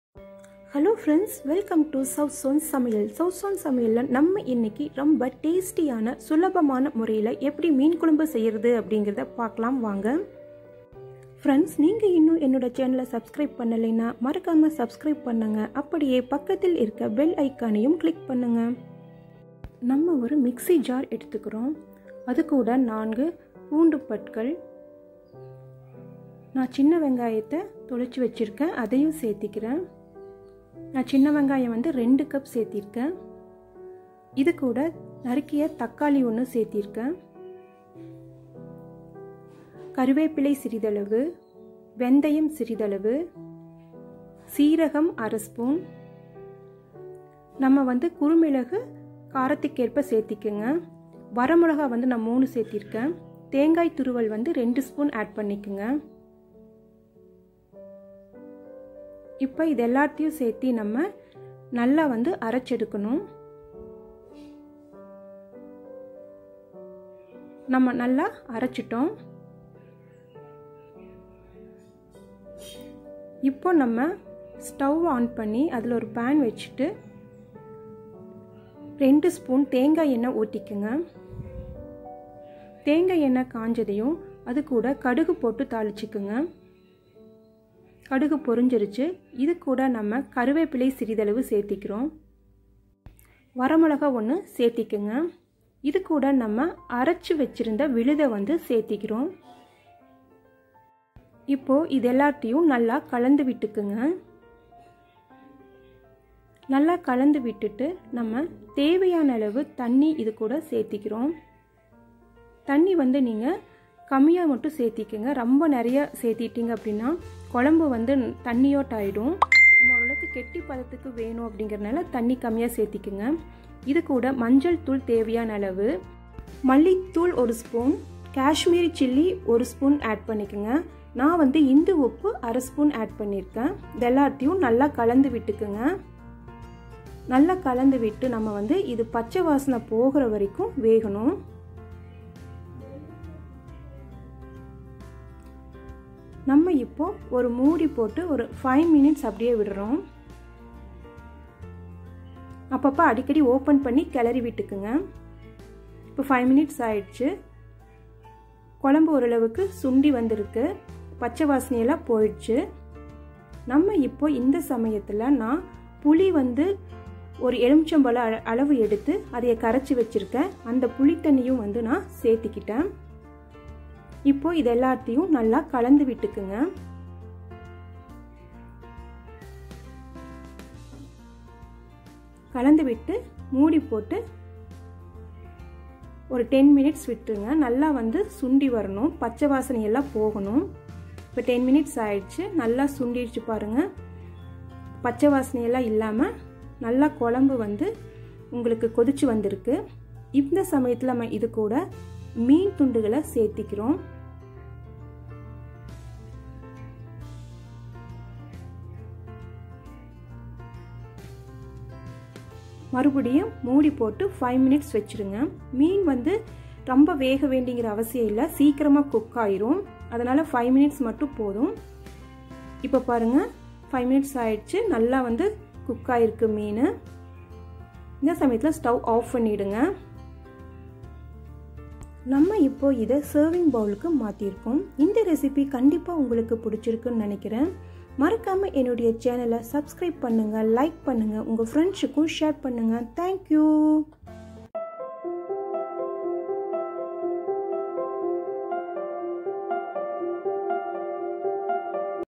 Meen kulambu in tamil||fish curry in tamil||மீன் குழம்பு ||south indian fish curry
in this video, we will show how to make meenkulambu very easily in tamil .south indian cuisine includes the recipes of the five southern states of India. this south Indian fish curry recipe also called meen kulambu,is  a delicately flavoured dish infused with tangy tamarind that can be served with idli,dosa,roti,chapattis and rice.

Ingredients

to make fish masala
garlic -4 pieces
small onions -2cup
fenugreek -1/4 spoon
black pepper -1/2 spoon
grated coconut -2spoon
curry leaves
kasmiri chilli powder -1spoon
coriander powder -1spoon
turmeric powder -1/2 spoon
rock salt -as required
tamarind paste
fleshed fish
coconut oil-2spoon